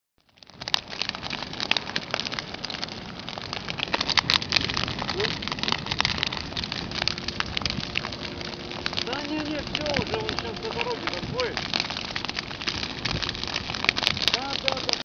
Kūlas dedzināšana
Neliels ieskats, kā izplatās ugunsgrēks, kad uzpūš spēcīgāks vējš. Mēģināt viņu dzēst ar rokām ir bezcerīgi.